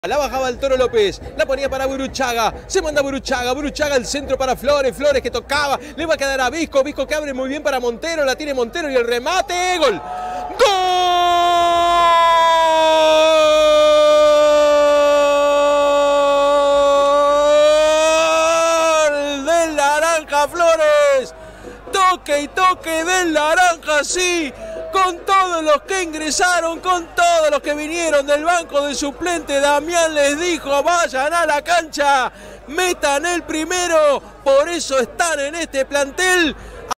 Alem 0-1 Berazategui / Gol / Primera C
Fecha 8 - Torneo Apertura - Primera C
Alem vs Berazategui
⚽GOL
43' (st) Matías Flores (BER)